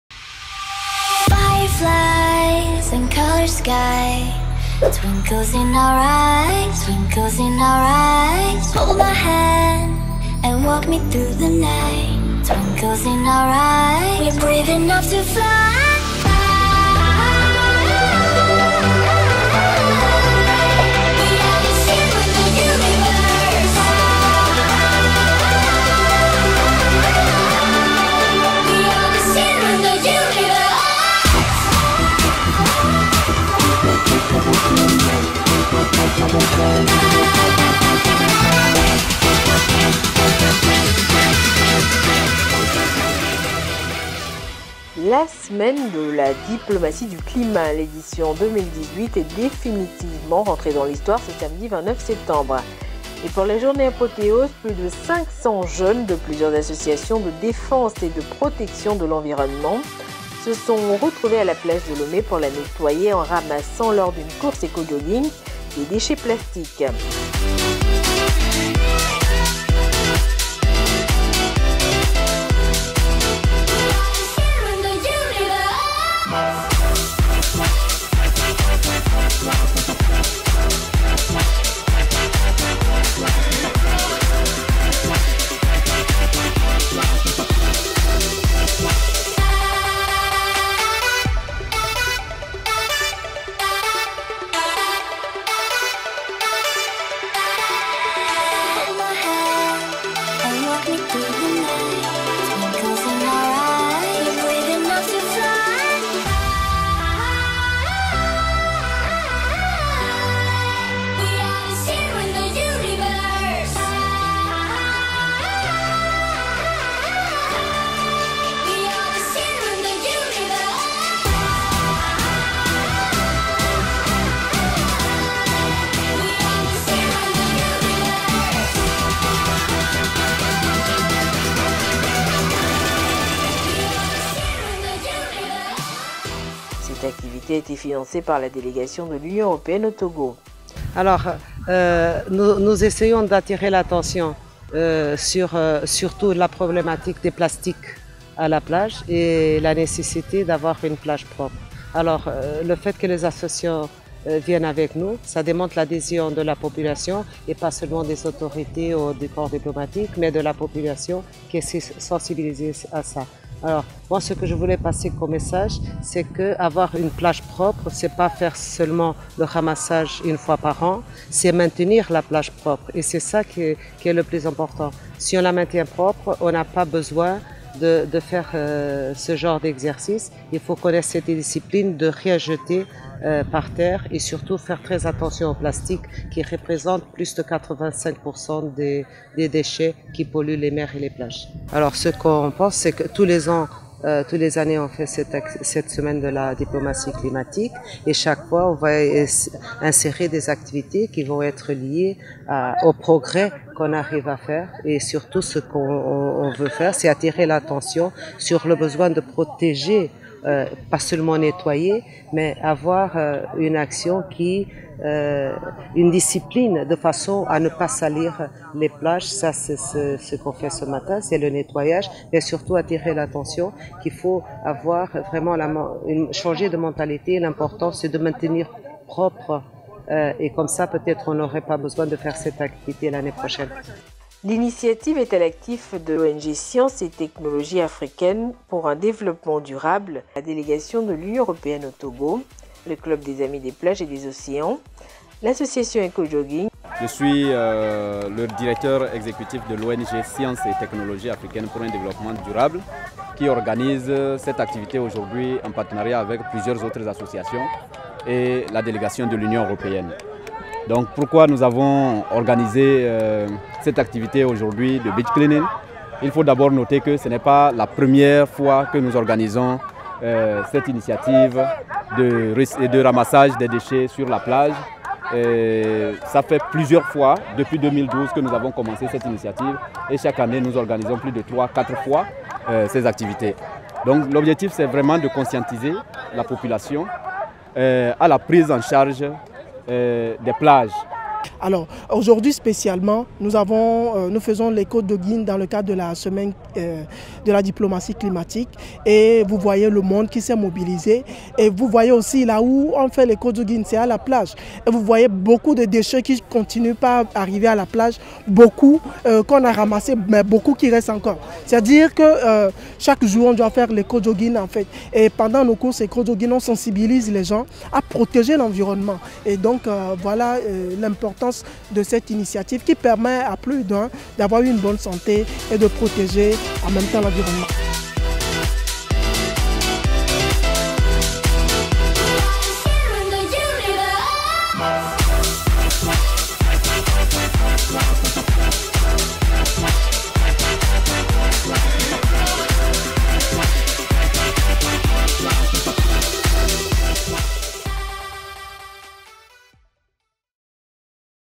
Semaine de la diplomatie européenne du climat - Beah cleaning 2018
L'Union européenne, conjointement avec CAPLO (Club des Amis de la Plage de Lomé) et STADD (Science et Technologie Africaines pour un Développement Durable), une ONG de défense de l'environnement, ont organisé le samedi 29 septembre 2018  une opération plage propre.
Cette opération a permis  de nettoyer des tonnes de déchets plastiques échoués sur le rivage.

L’exercice, baptisé "Plage propre 2018"  faisait partie d'une campagne mondiale de l'Union européenne visant à réduire le danger que représentent les déchets plastiques pour l'océan et visait à maintenir la propreté des plages tout en sensibilisant l'opinion publique aux effets néfastes de la pollution sur notre environnement.